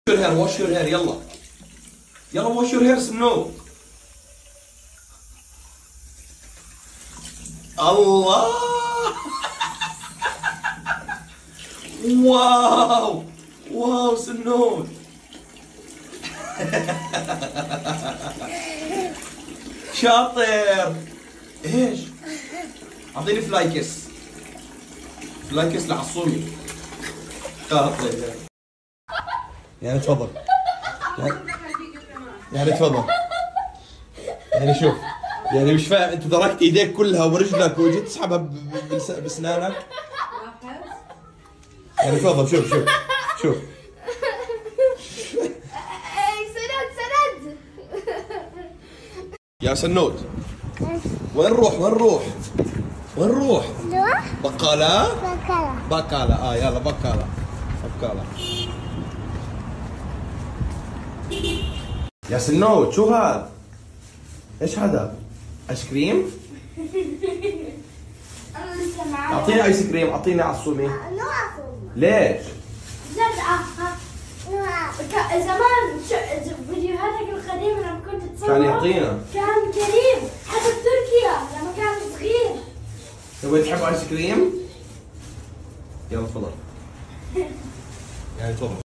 عصومي مقداد سند بيأخذ شور في الصباح😂عصومي مقداد بده آيس كريم من سند 😂😂🍦🍦☝️☝️
عصومي مقداد سند بيأخذ شور في الصباح وعصومي مقداد بده آيس كريم من سند 😂😂